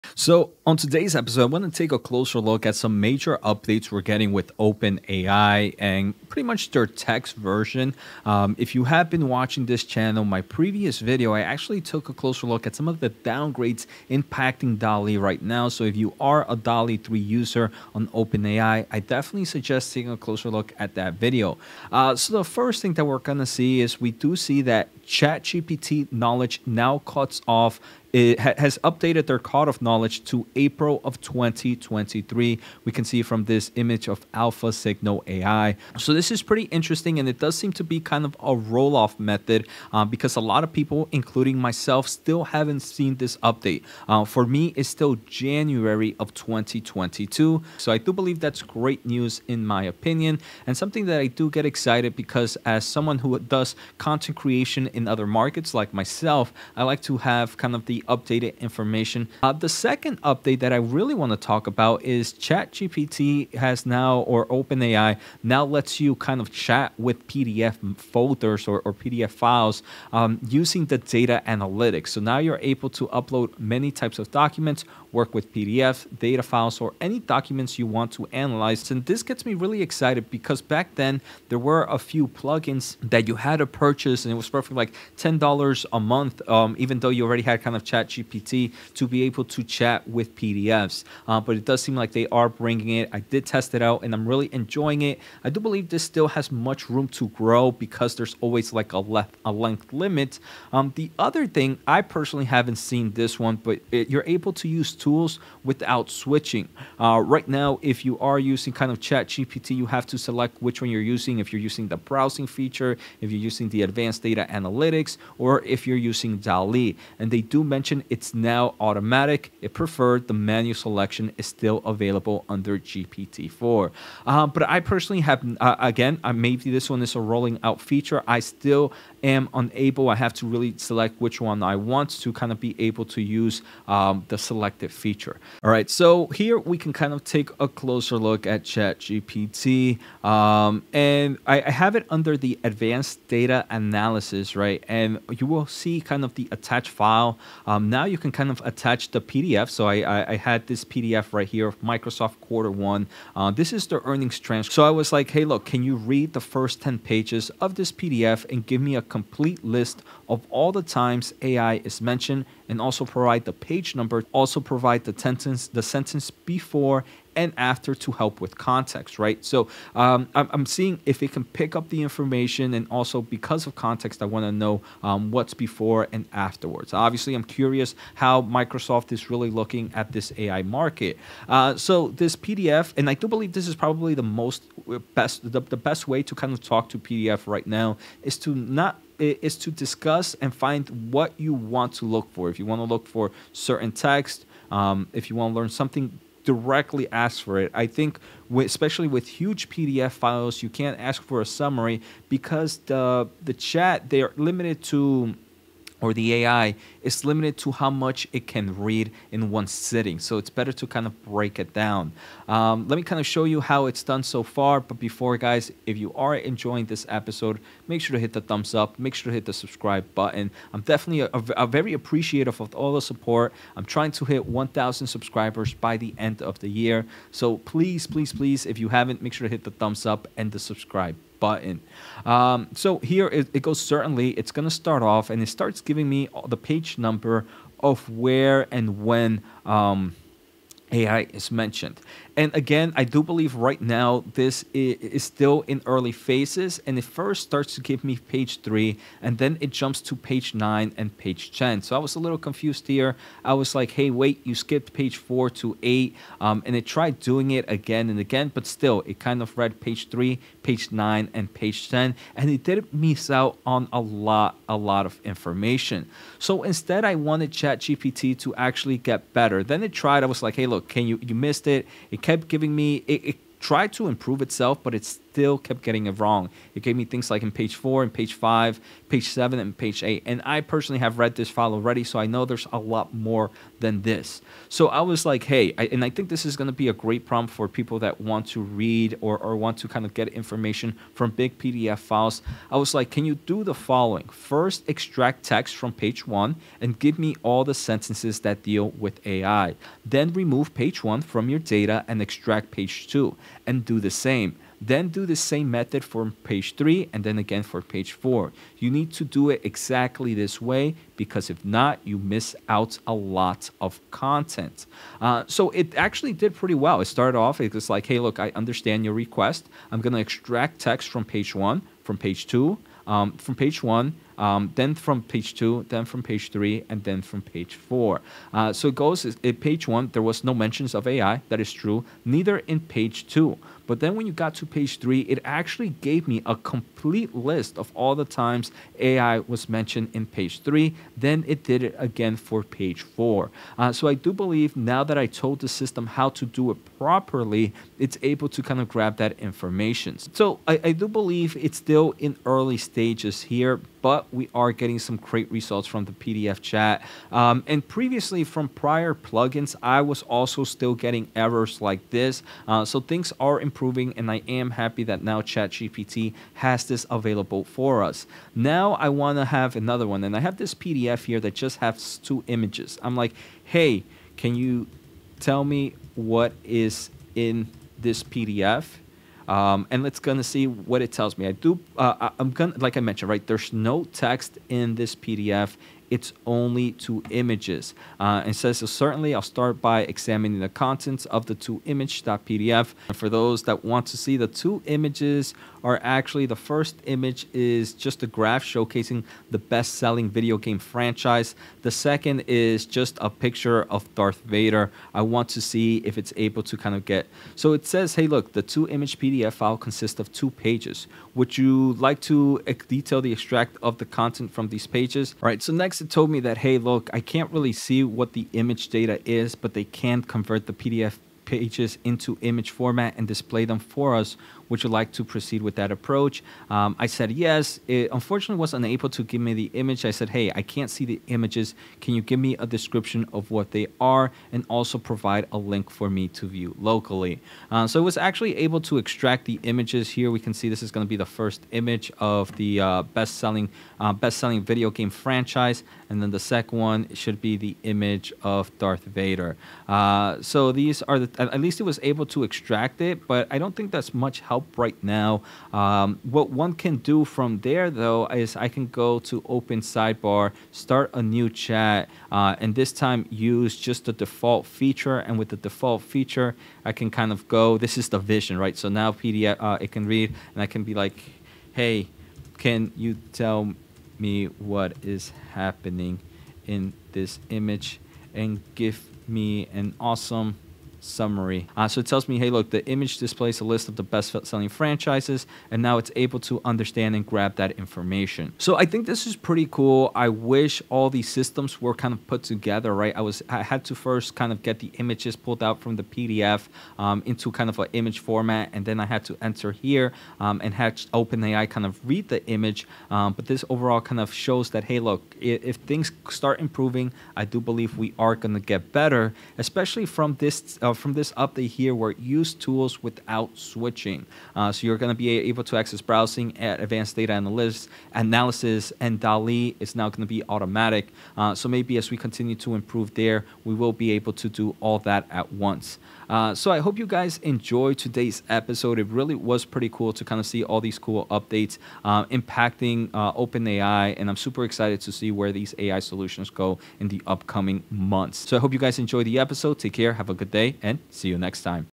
ChatGPT AI Just Got SMARTER With New Update -- AI PDF
OpenAI's CHATGPT got a new update where the AI can read PDF documents and answer questions. Is ChatGPT OPENAI the top AI tool to use right now?